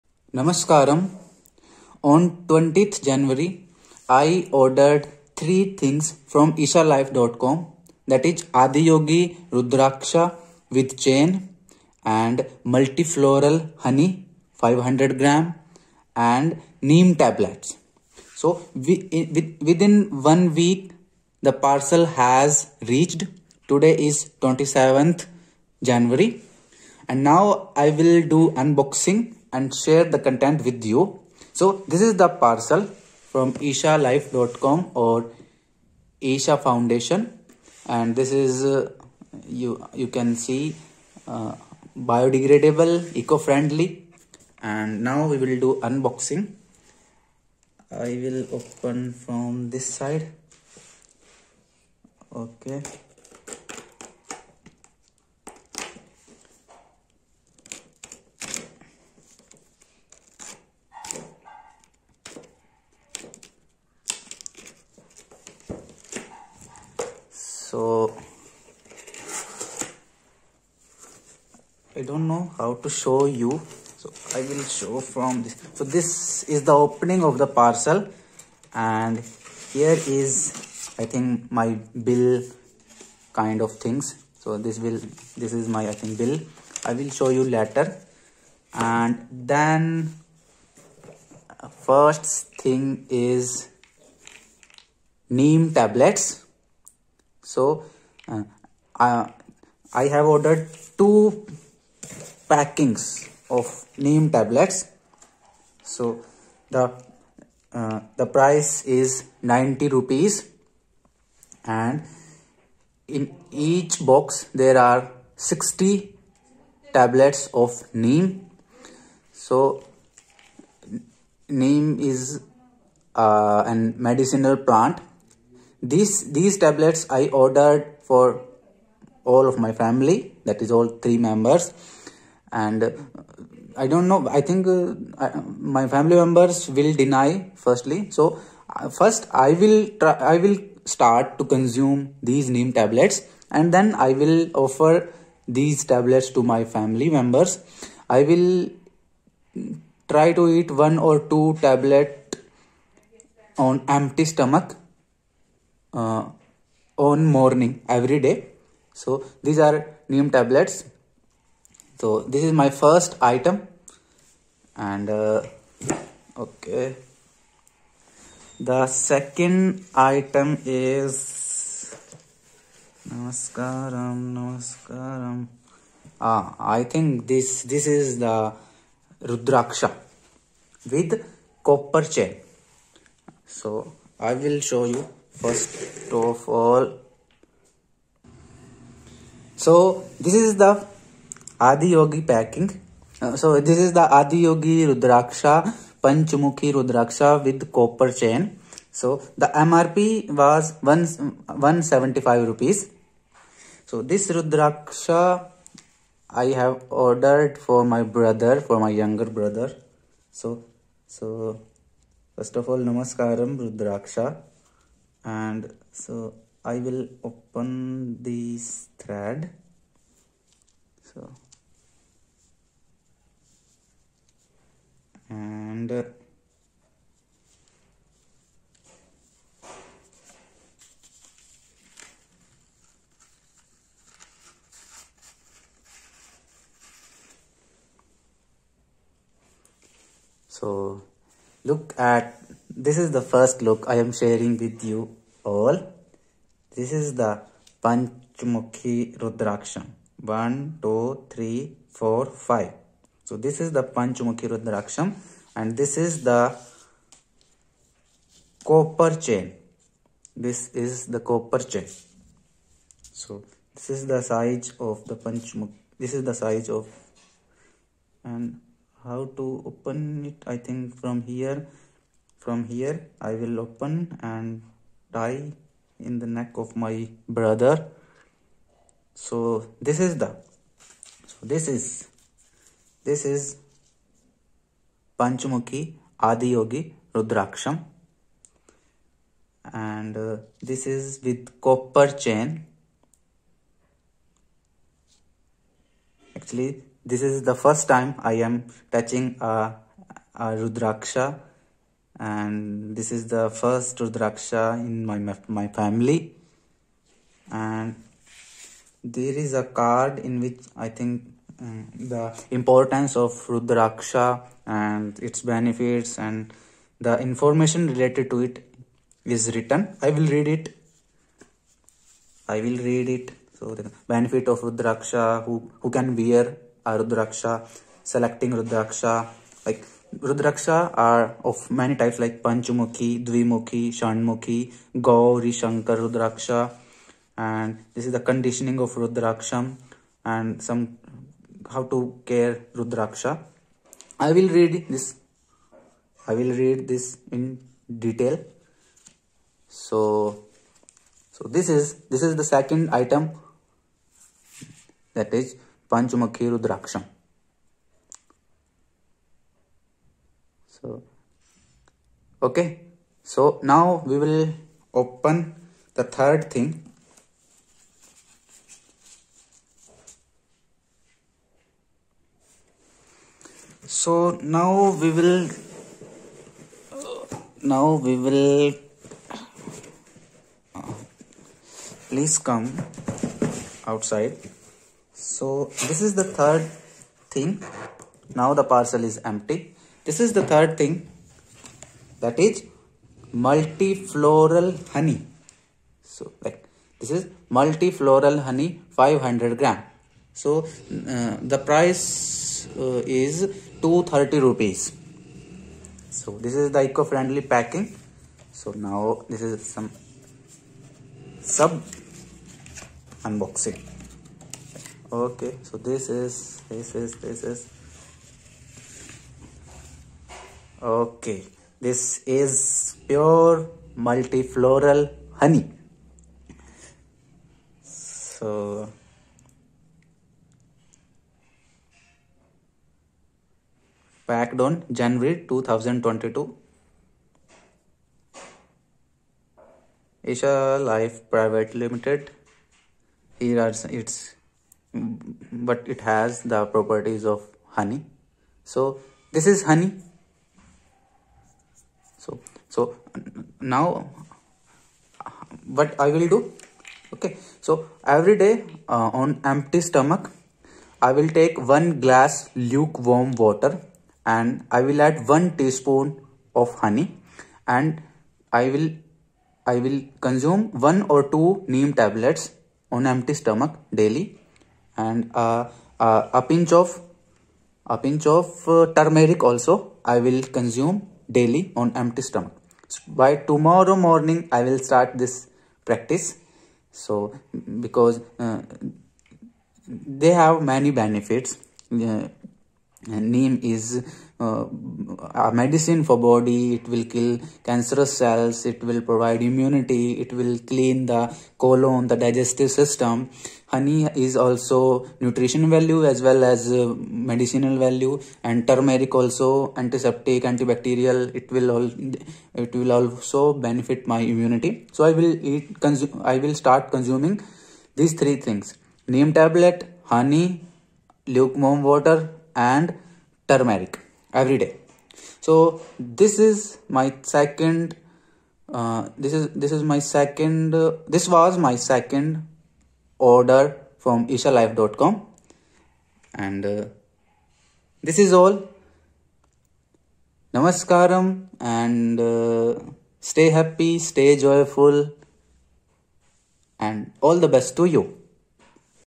Unboxing of Adiyogi Rudraksha with Chain, Neem Tablets and Pure Multifloral Honey
Namaskaram 🙏. In this video I am doing unboxing of Adiyogi Rudraksha with chain, Neem tablets and Pure Multifloral Honey. I ordered these items from ishalife.com

Unboxing
Rudraksha
Sadhguru Rudraksha Diksha
Neem medicine
Honey
Pure Honey
Medicinal Honey
Ishalife
Ishalife products unboxing
Sadhguru Isha Foundation
Isha Foundation